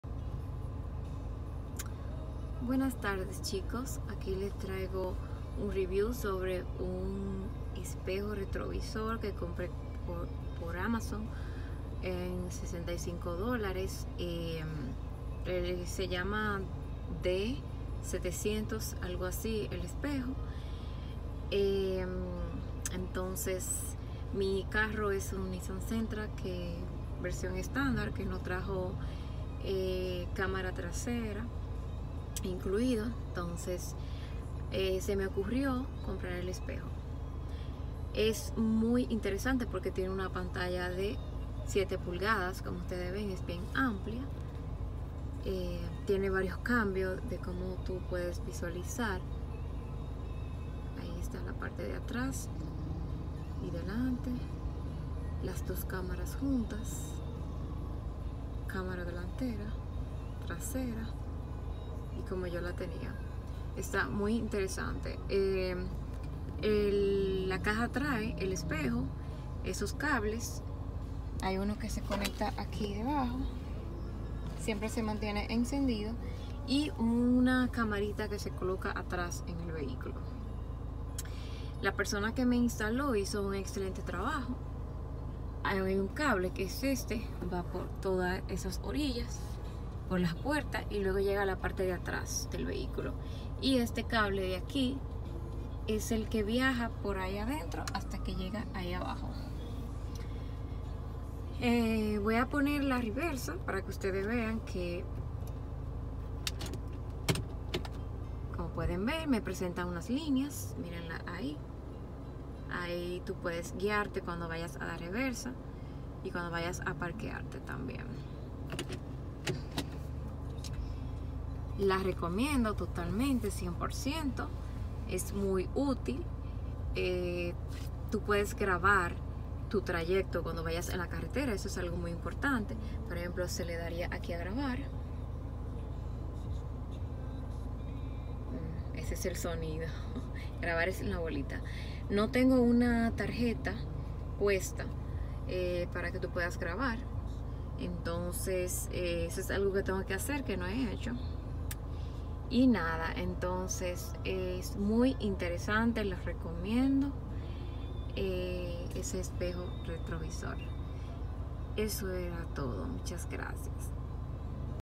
Pruveeo D700 espejo retrovisor touch cámara de reversa
Espejo retrovisor touch con cámara de reversa pantalla 7 pulgadas con cámara delantera que permite grabar y cámara trasera de reversa con buena visión.